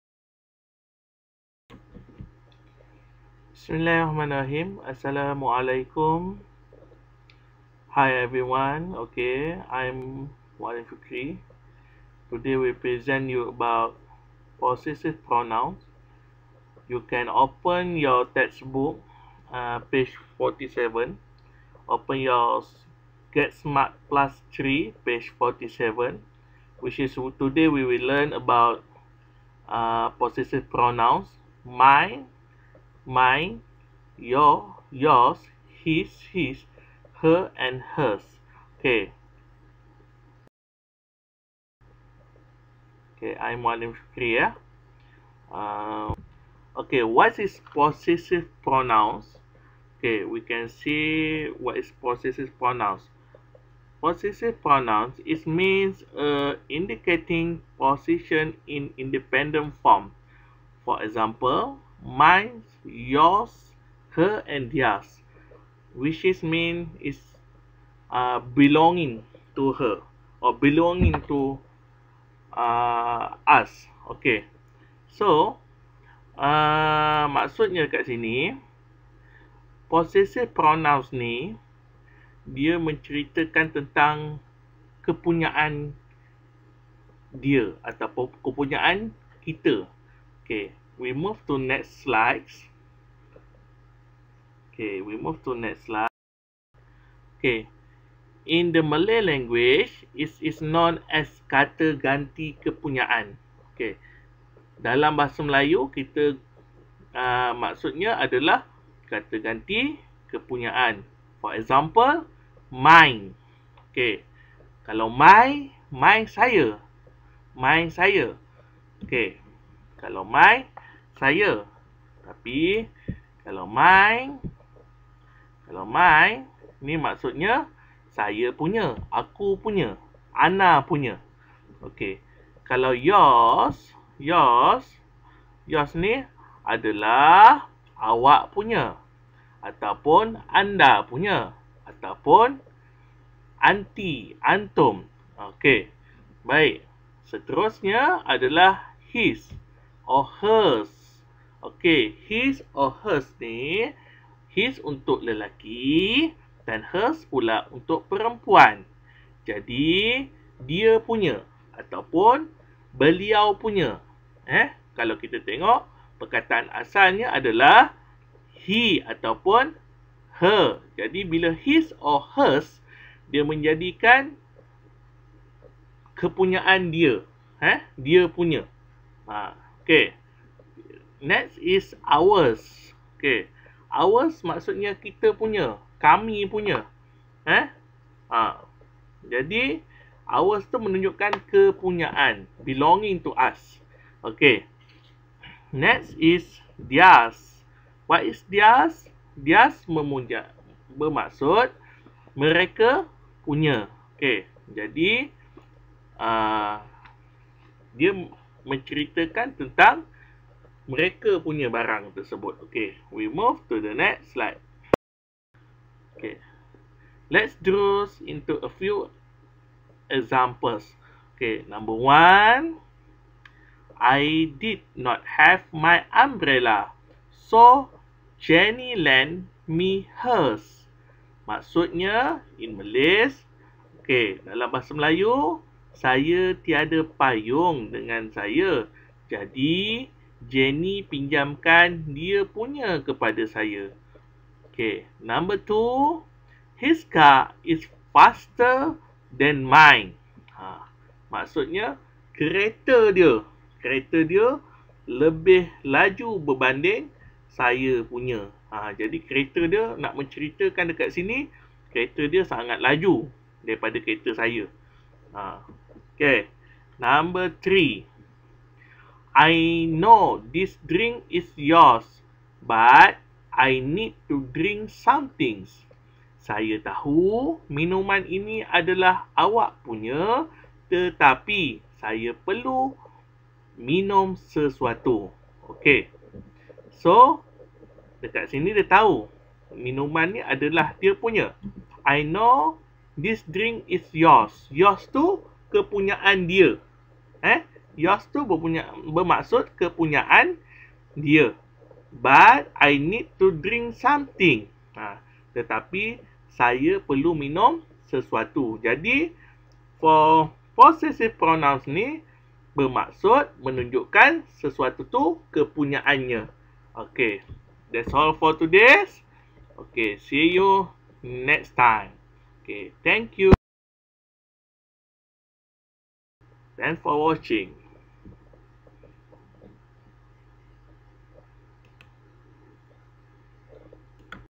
Possessive Pronouns
Short video explaination about possessive pronouns for year 3 Get Smart Plus 3 module 5 page 47...